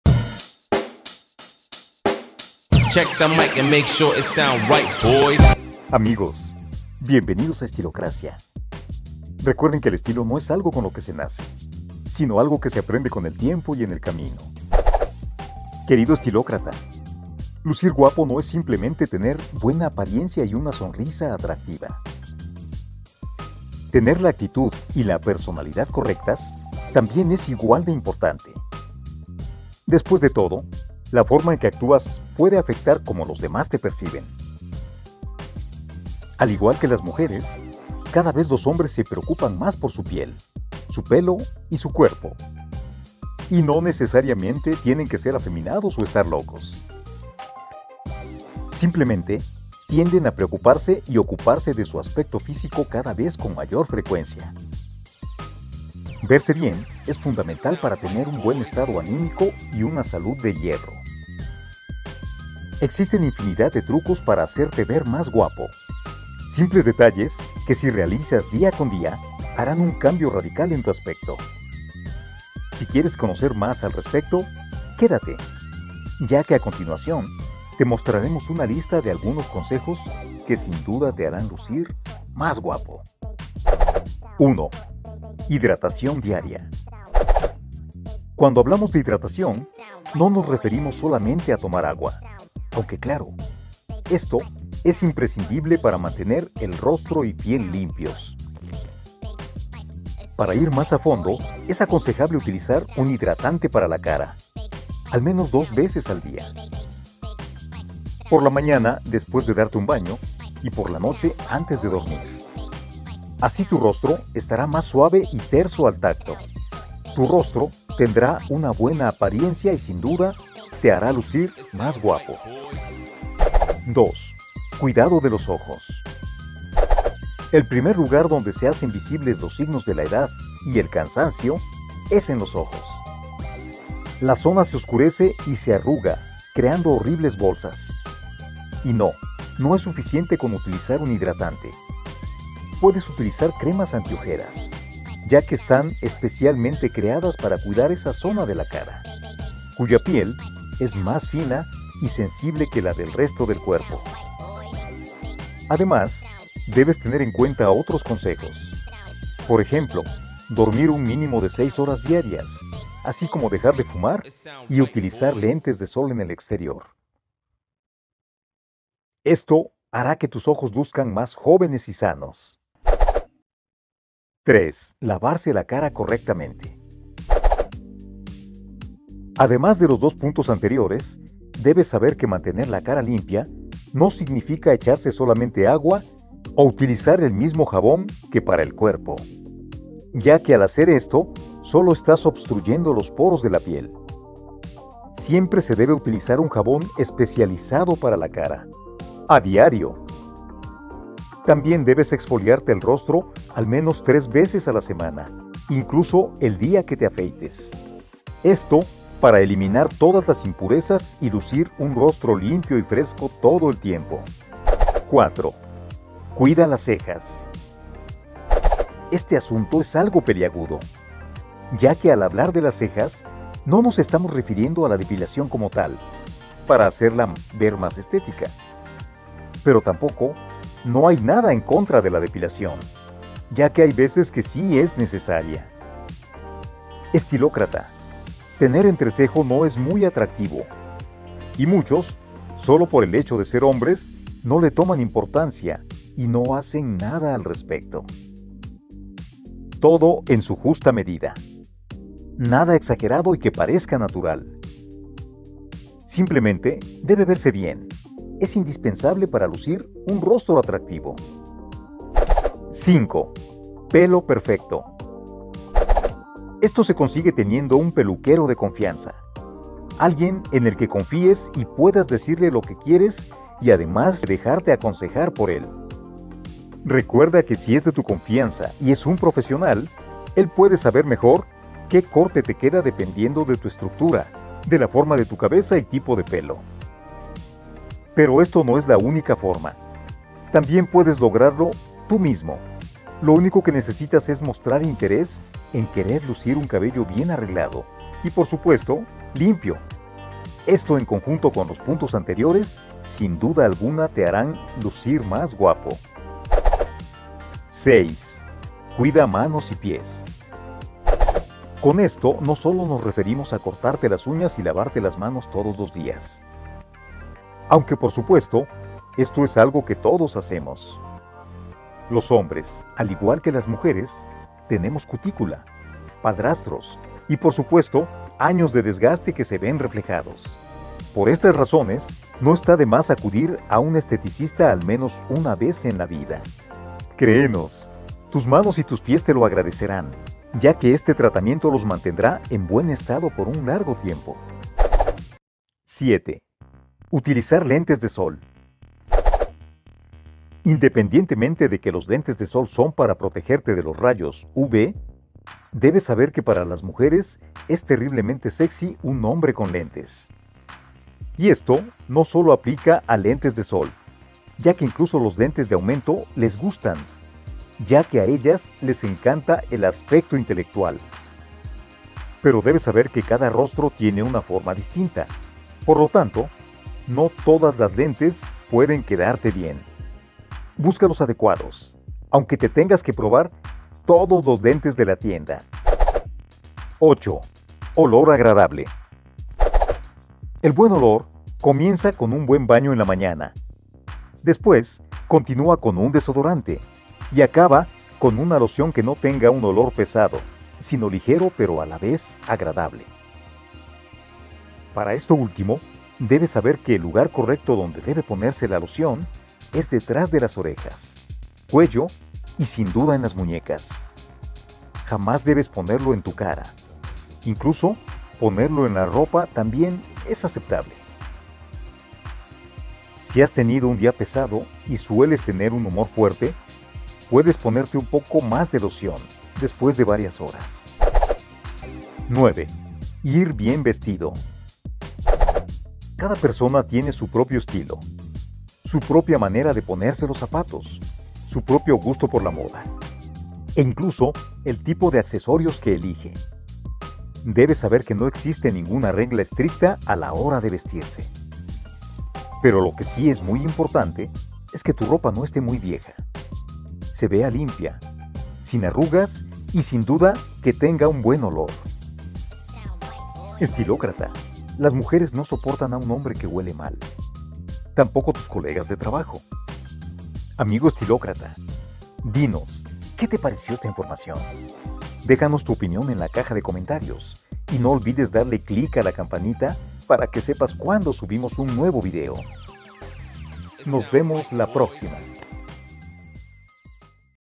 😎 CÓMO VERSE MENOS FEO: 7 TRUCOS y HÁBITOS FÁCILES para aplicar SER GUAPO!!
SABÍAS QUE CON ESTOS TRUCOS PUEDES VERTE MEJOR MAÑANA MISMO!

Visita nuestro sitio asociado de Amazon para productos de tu interés: